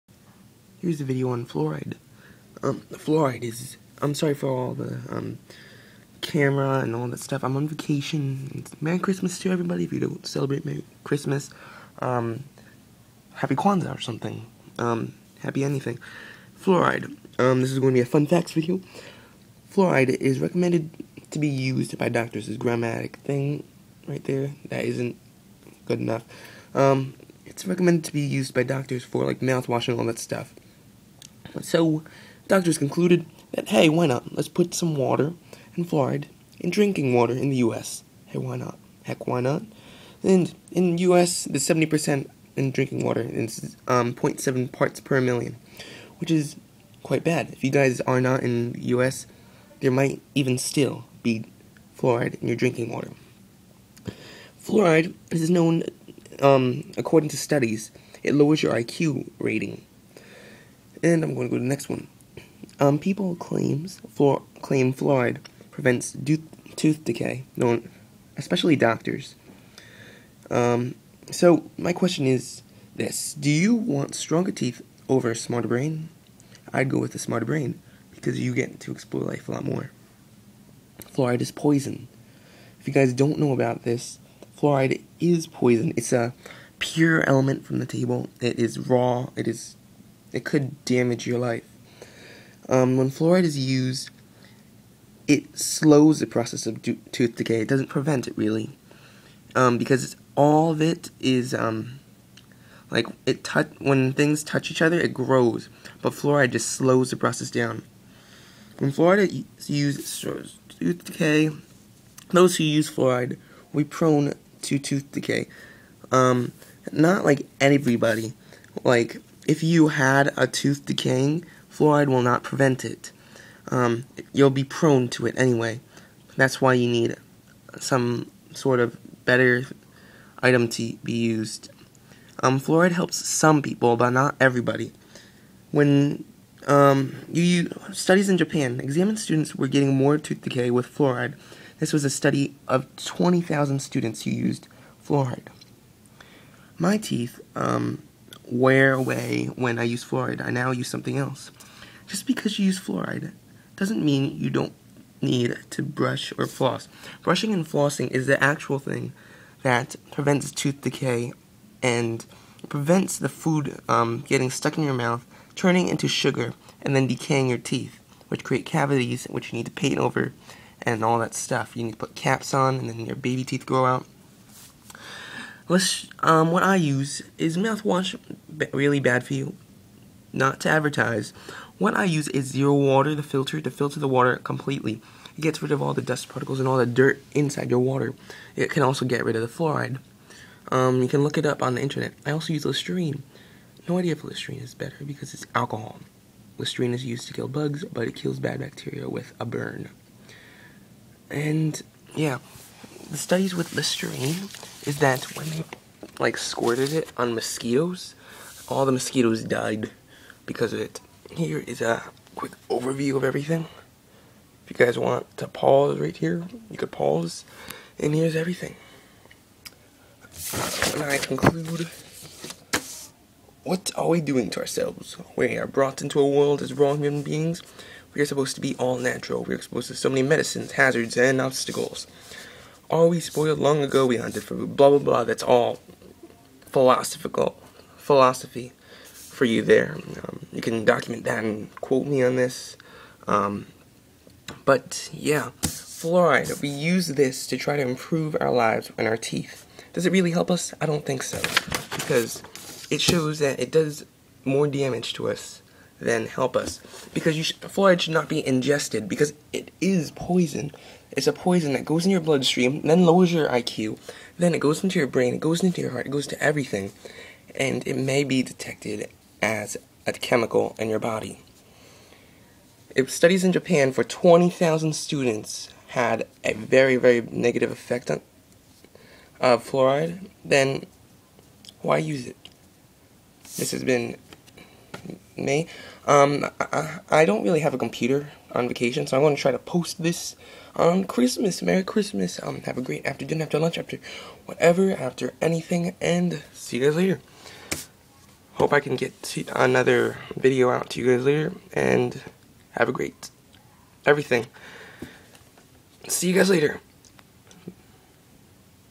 Random Facts - Drinking Fluoride Lowers IQ Rating - Ep2
Fluoride lowers the brain iq.  When you used fluoride as a mouth wash it is most likely not helping.  I've made a few studies to educate you all about fluoride no matter how much it may change your lives.